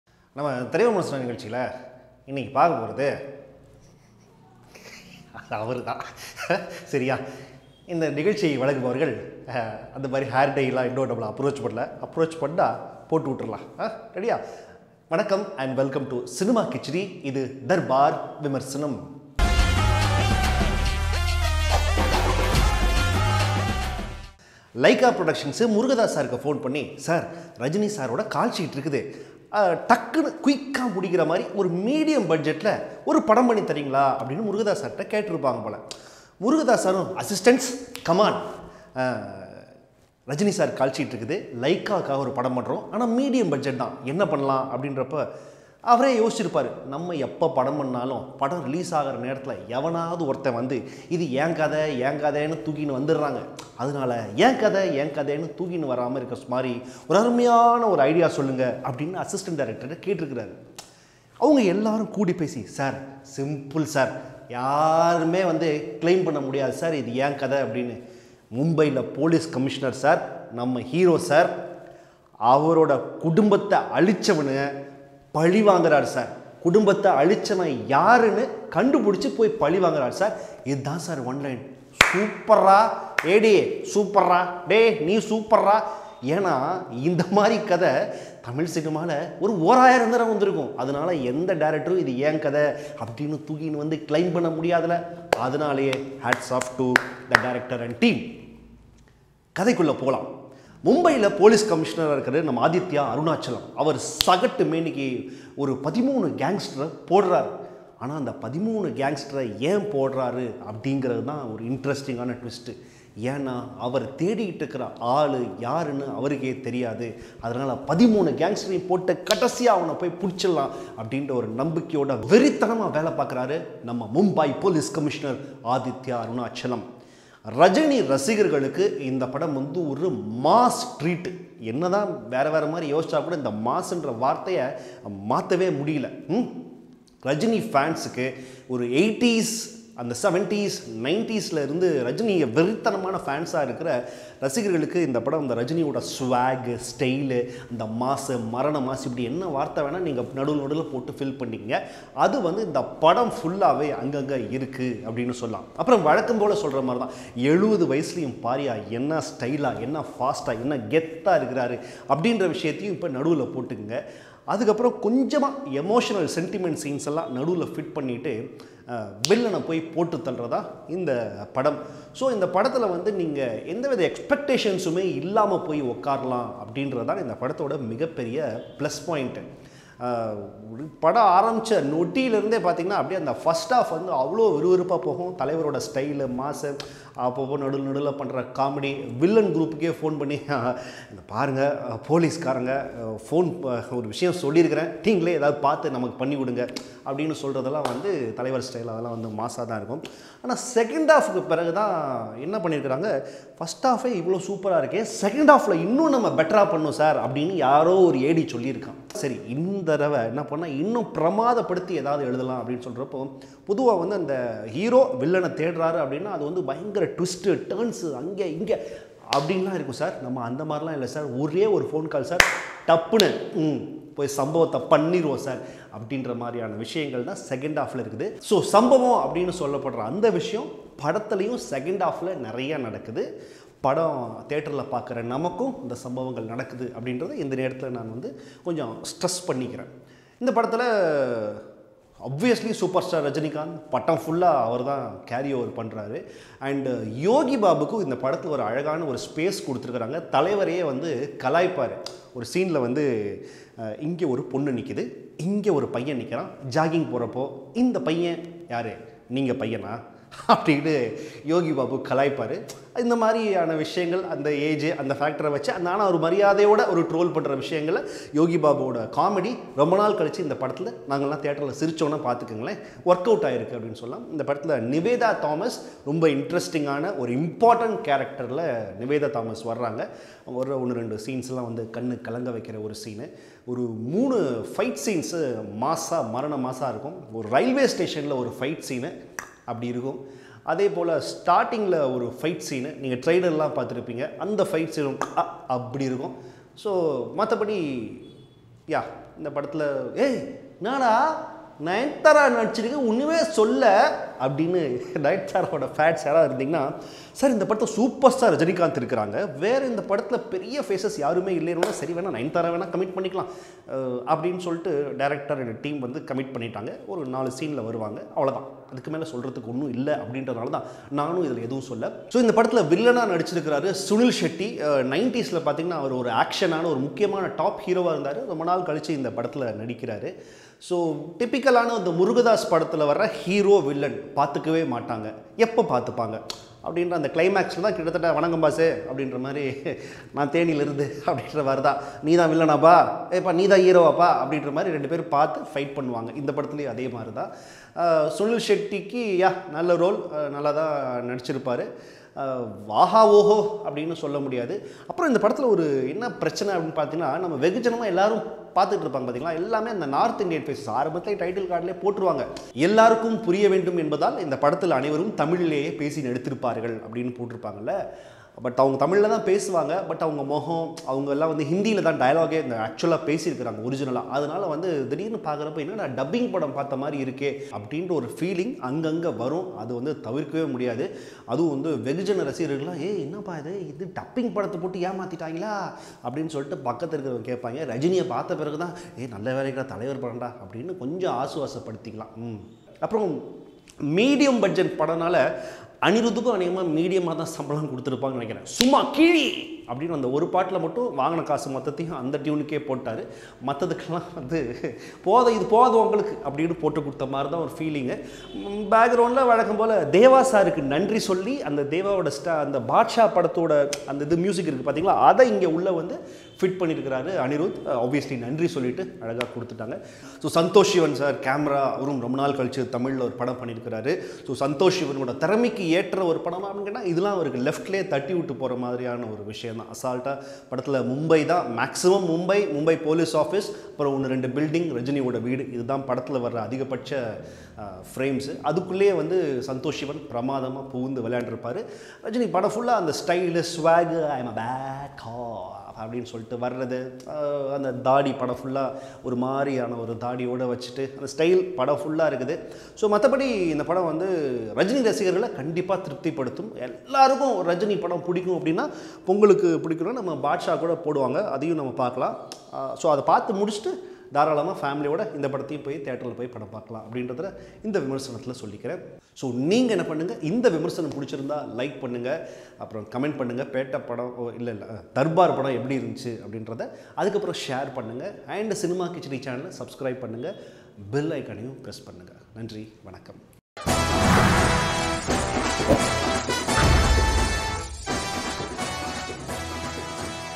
Darbar Review by Randy|Rajinikanth|AR Murugadoss|Nayanthara|Kichdy
Here is the movie review of darbar Starring Rajinikanth and Nayanthara
padam eppadi irukku
Follow us @twitter.com/kichdy
Like us @fb.com/kichdy